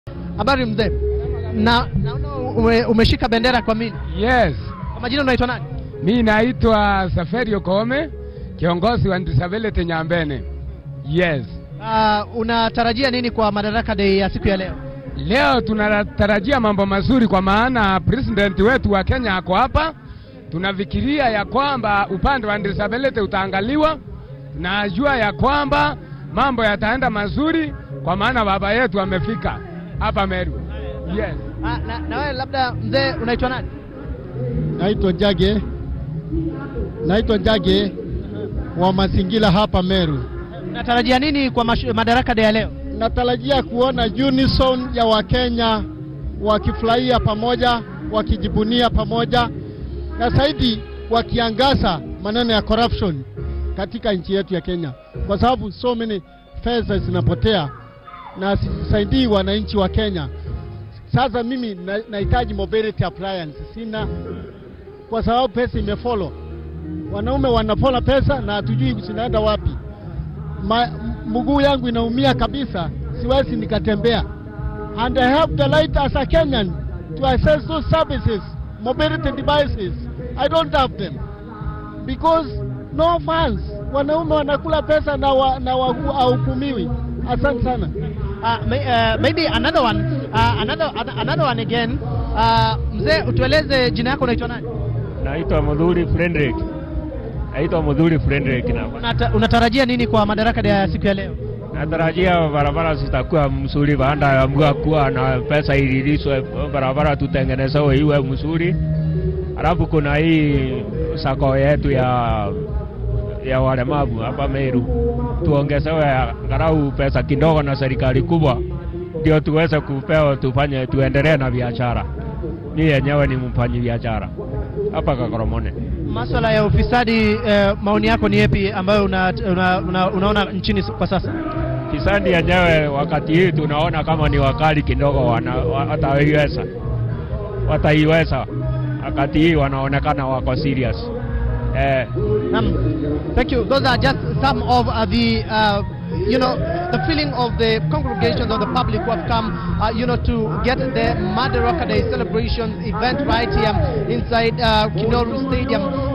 Kenyans expectations during the 55th Madaraka Day celebrations
The president wil lead the celebration at Kinoru stadium Meru